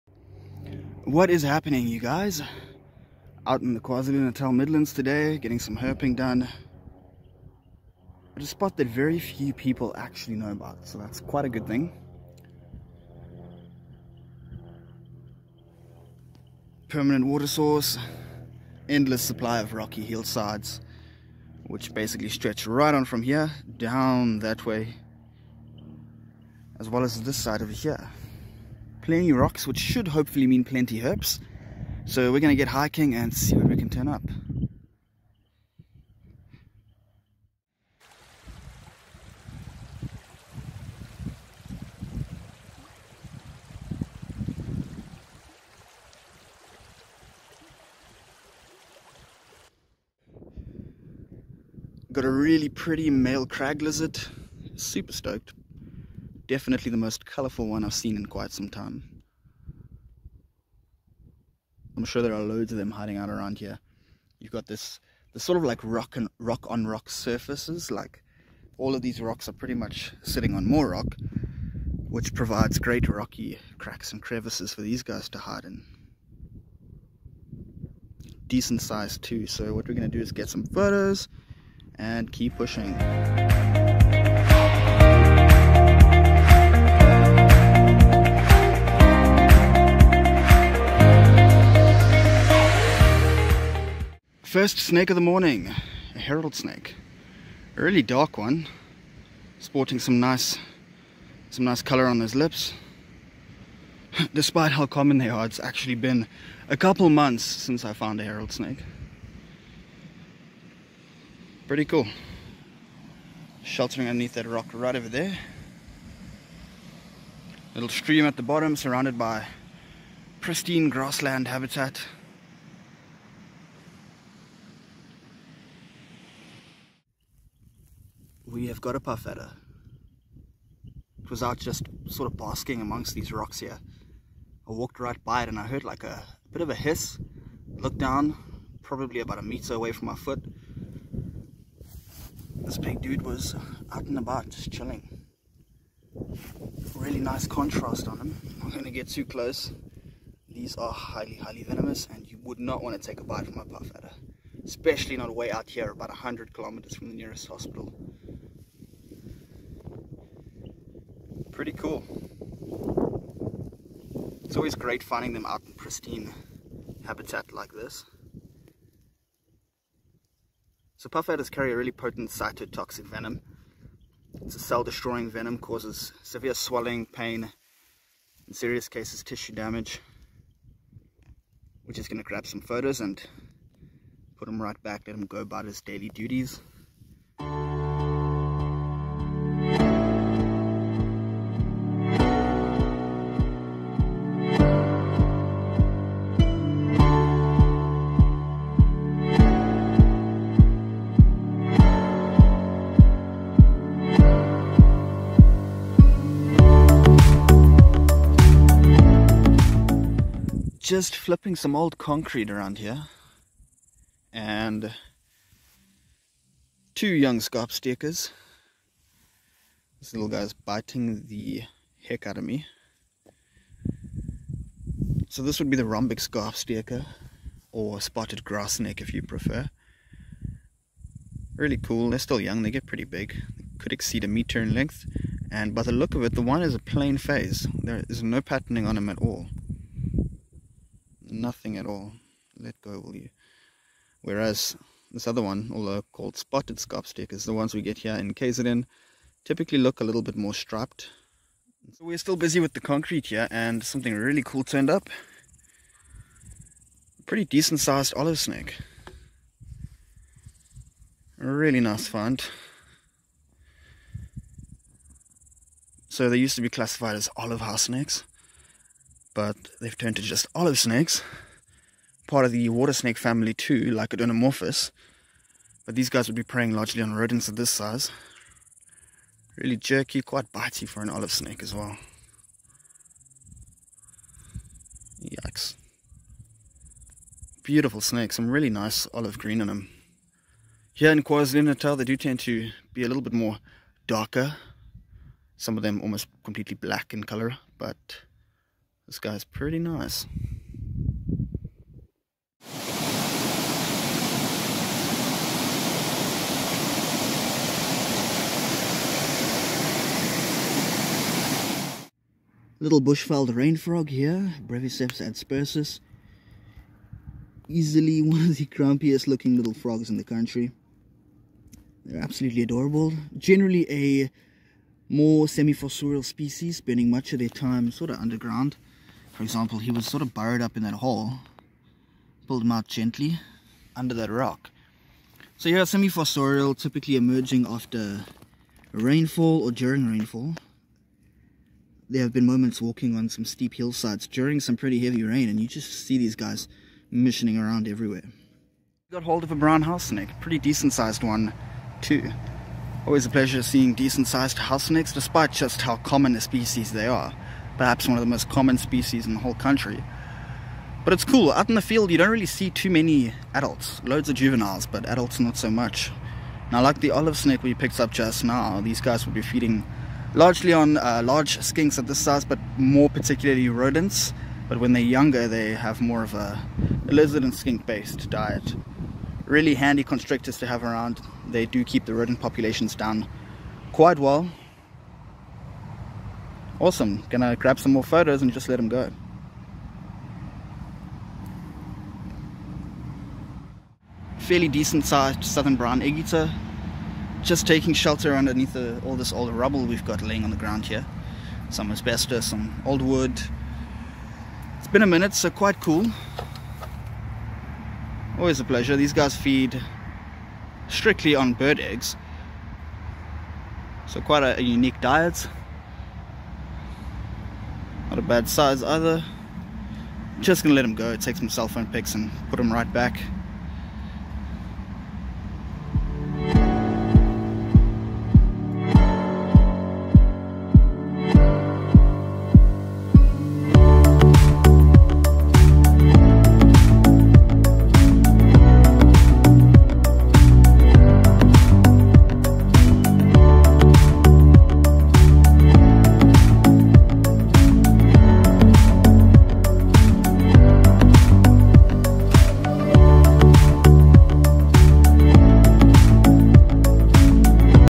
DEADLY SNAKES and COLORFUL LIZARDS! Midlands South Africa
Another day out in the KZN Midlands searching for reptiles and amphibians. Until next time.